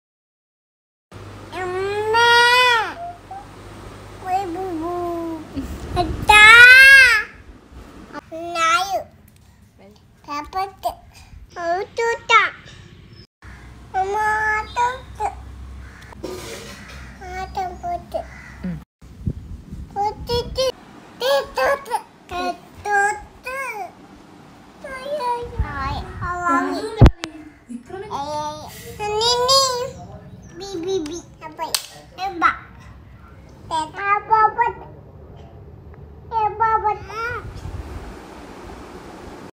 വർത്തമാനം പറഞ്ഞു തുടങ്ങ്യപോ. Poko's Baby Accents
OMG! Your baby accent is so Cute.. SAY IT AGAIN !!

I just smile or blush whenever I see this, just like I see him.
These words turned to sentences now.